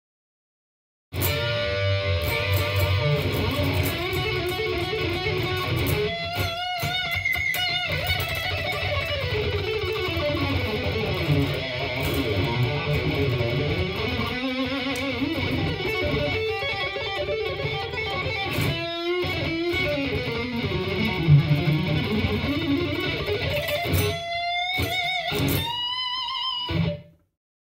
iPhone SE Camera Test (Audio - Video) - Electric Guitar Solo Shredding 4K UHD
This is a short Camera test of my new iPhone SE.

Equipment:

►Video: iPhone SE
►Guitar: LTD Eclipse EC-1000
►Sound: Logic Pro X, Amplitube
►Audio Interface: Tascam US-366